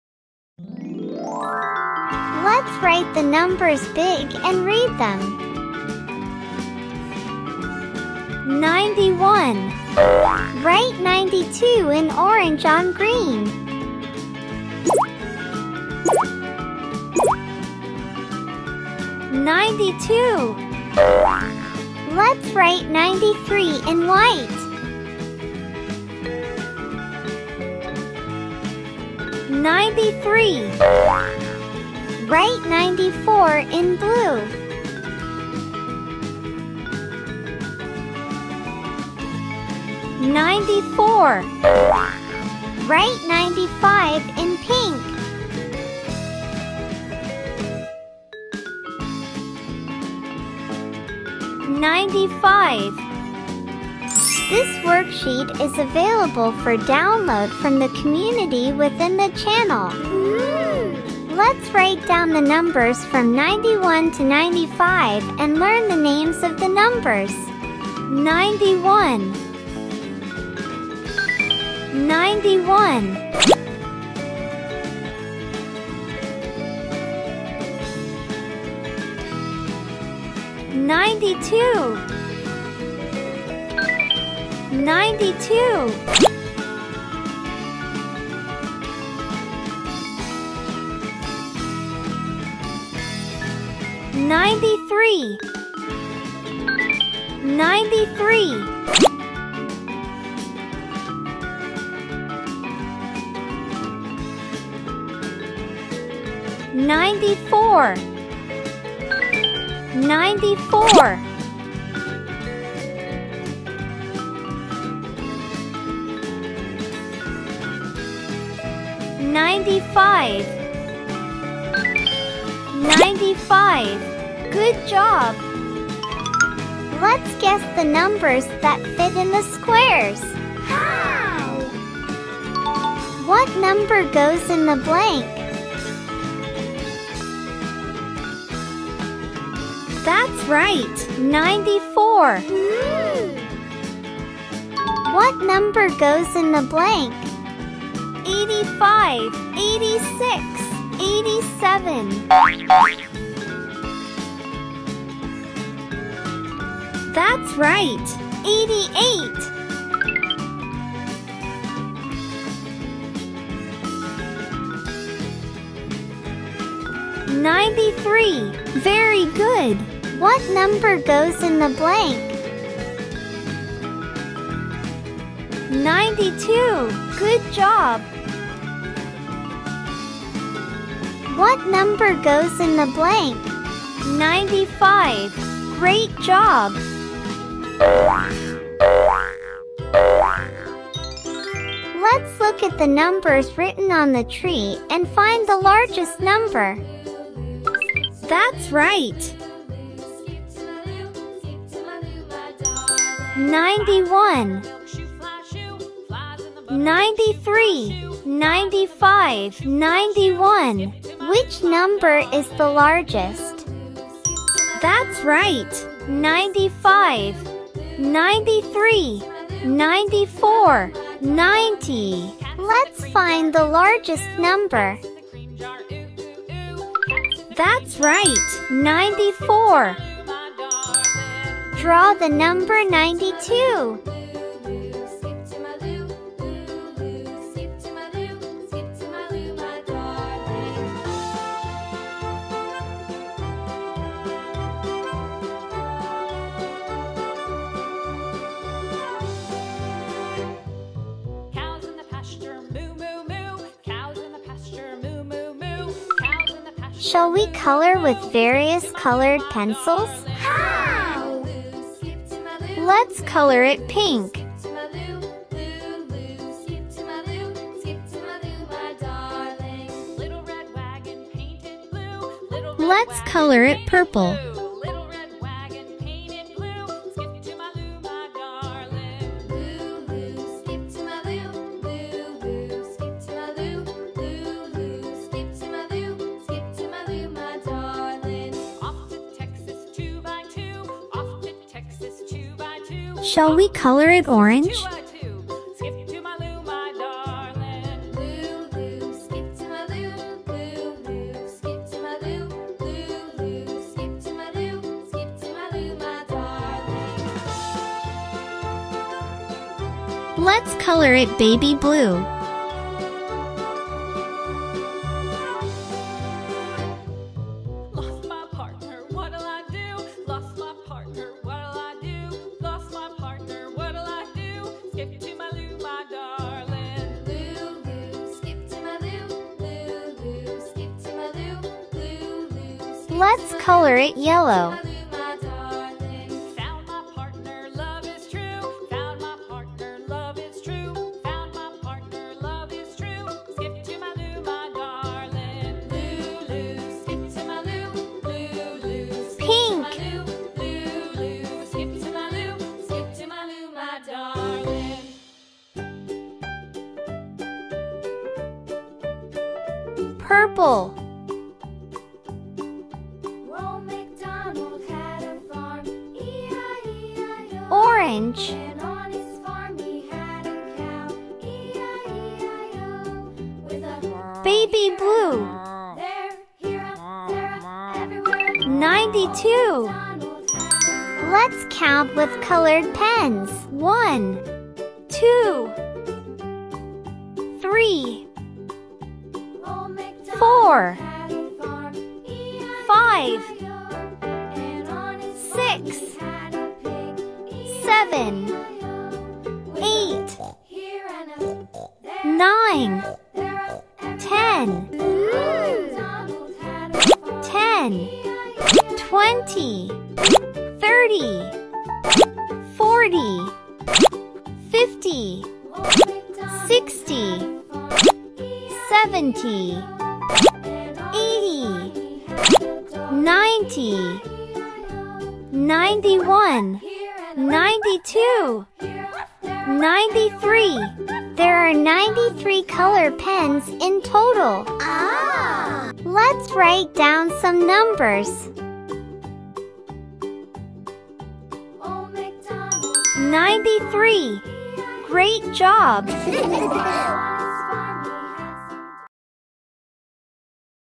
Learn the names of numbers from 91 to 95 | Learn numbers by finding the largest number
Hello!
Today, let's learn numbers from 91 to 95.
Let's learn in a fun way by writing and reading numbers in various colors on a colored background.
Write the numbers along the dotted lines and learn the names of the numbers.
Learn the numbers by guessing which number goes in the blank and practicing finding the largest number among three numbers.
Draw the numbers with colored pens and learn the shapes of the numbers by coloring them with colored pencils.
Learn how to count numbers by counting the colored pens one by one and learn the numbers in various ways.

You can download the worksheets from the community.
The learning effect will be greater if your child follows along and watches the video.